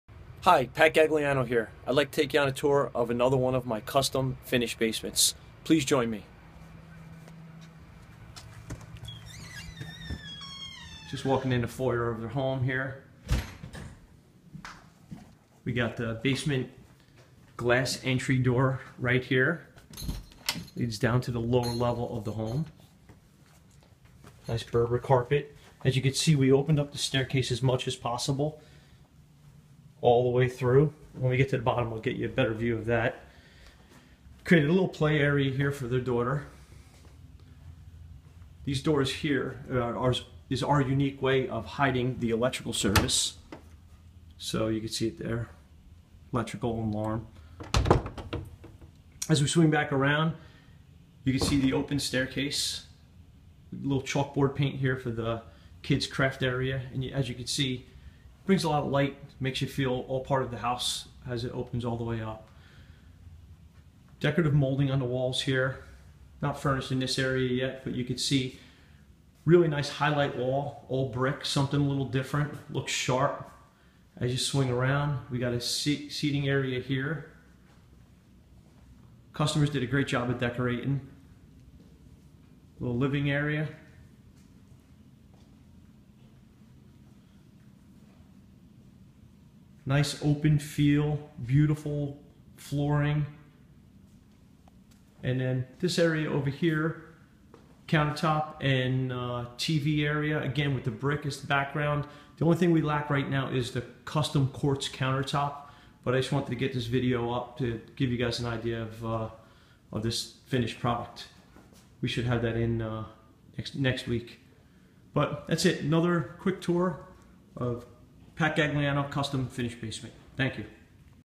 Finished Basement with Brick Feature Wall
Very cool finished basement with brick feature wall, sitting room, play room and open steps with iron railings.

Follow our Instagram - @FinishedBasementNJ
and our Facebook - Finished Basement NJ